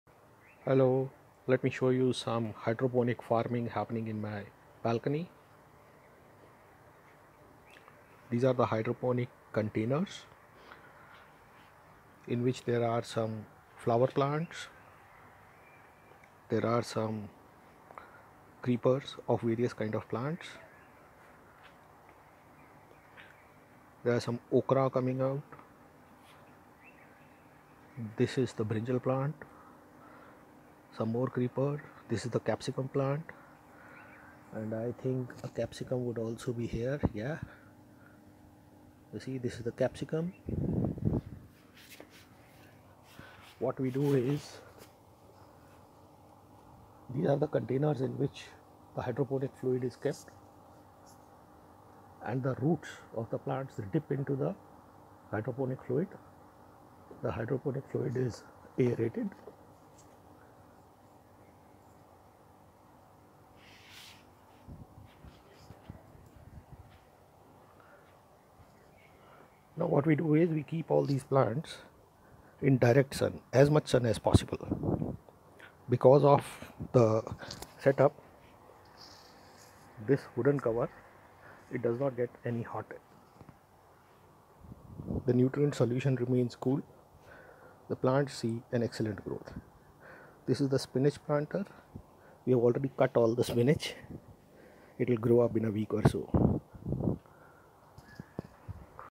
Balcony Hydroponic systems
Its very easy to practice hydroponics in balconies. You can easily grow leafy & vegetable plants in your balconies, where sunlight is available. Key is to keep the system simple, for easy management. Use GreenLoop Hydroponic nutrients for maximum growth. Use soft water to make the hydroponic solution.
Most people buy complicated hydroponic systems, then find it difficult to manage them. We recommend simple Deep Water Culture (DWC) systems for home use.